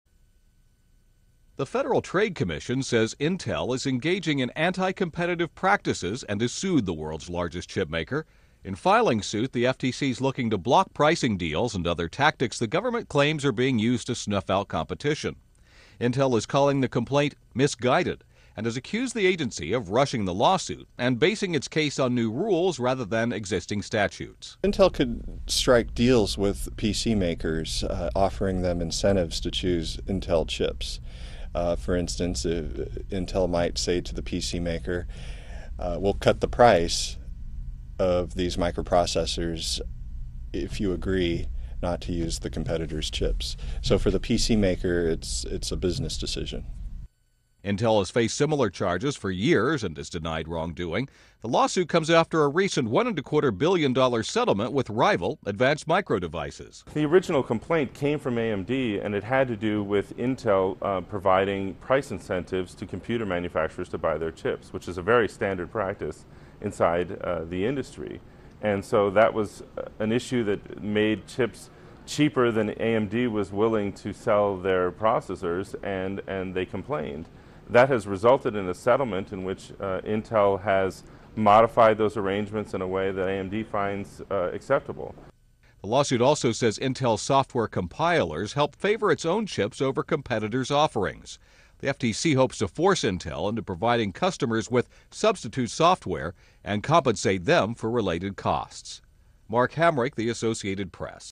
FTC Accuses Intel of Stifling Competition
The Federal Trade Commission has sued Intel, the world's largest chip maker.In filing suit, the FTC is looking to block pricing deals and other tactics the government said the company has used to snuff out competition. (Dec. 16)